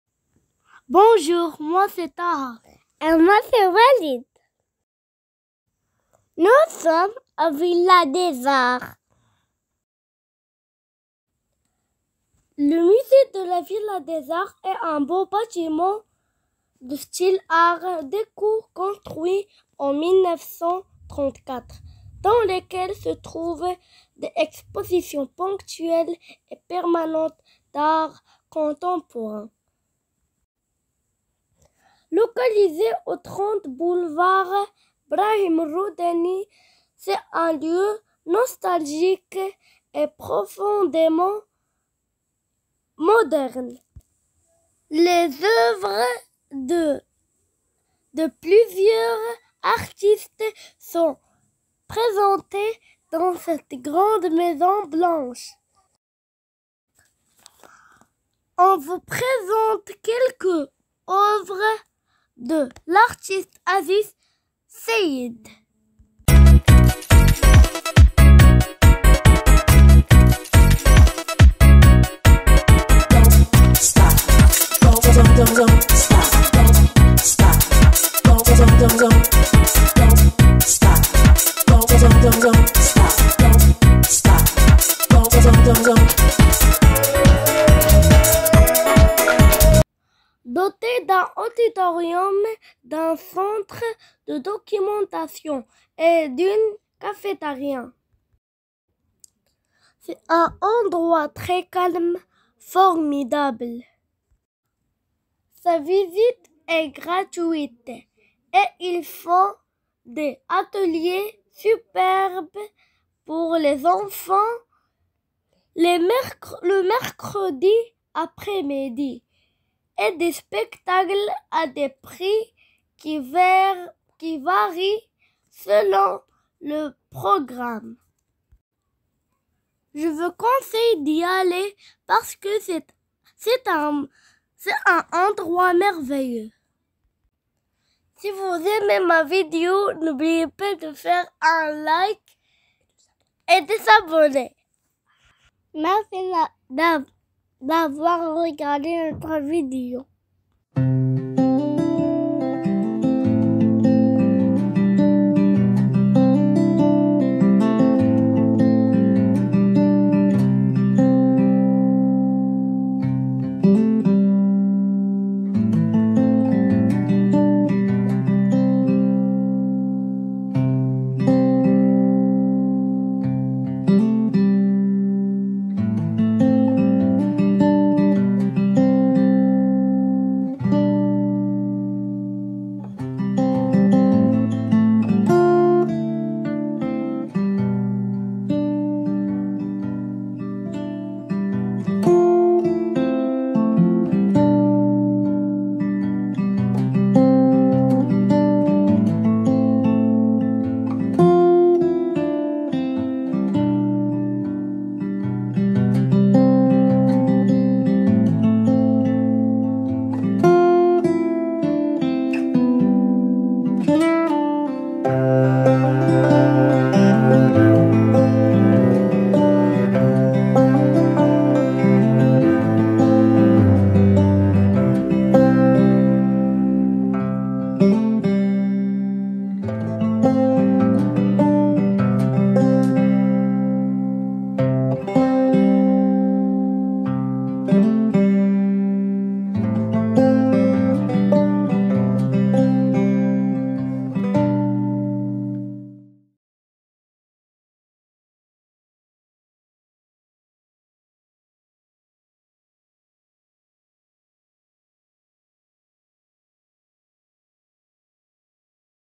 villa des arts à Casablanca
visite au musée de la villa des arts sur casablanca ou en a vue les œuvres de l'artiste Aziz Sayed , l'auditorium et plein de magnifiques toiles.